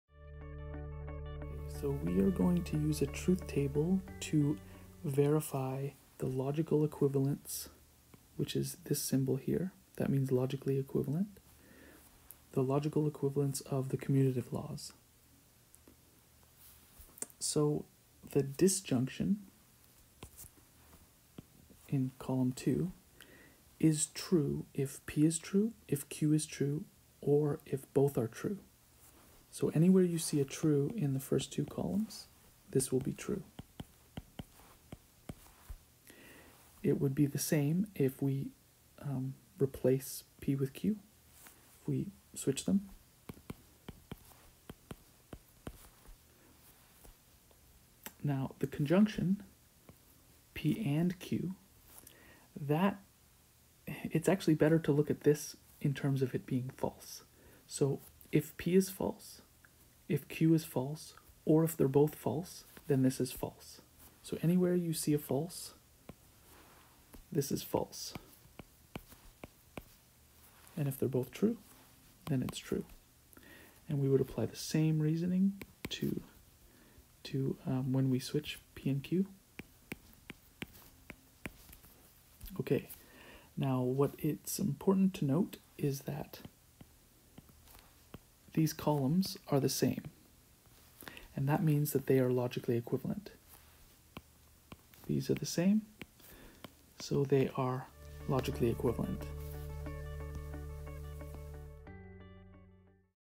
Propositional logic - Verify commutative laws with a truth table
This video shows how to use a truth table to verify the commutative laws of a conjunction and disjunction.